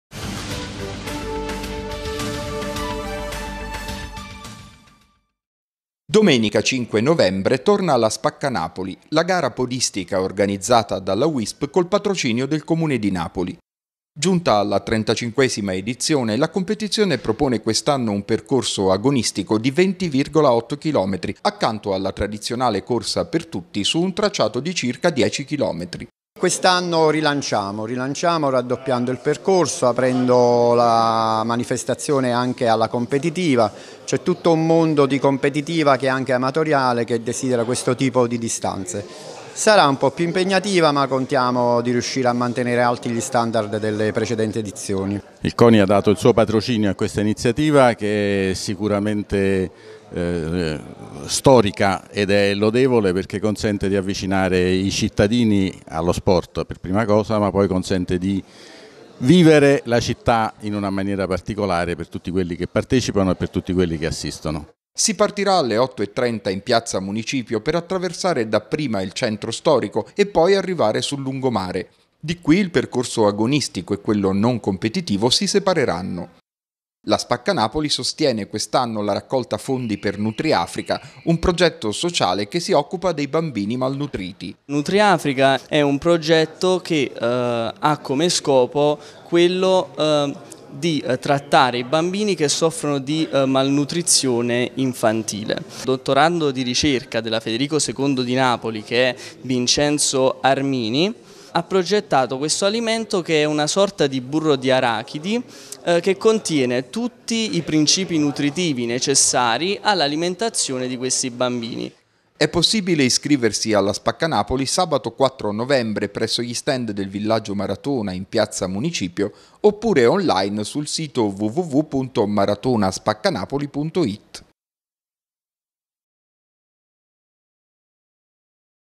La SpaccaNapoli raddoppia: si corre per divertirsi e anche per gareggiare
Appuntamento domenica 5 novembre alle 8.30 in piazza Municipio per la trentacinquesima edizione della SpaccaNapoli, gara podistica con un percorso agonistico (20,8 km) e uno non competitivo (10 km). Informazioni e iscrizioni su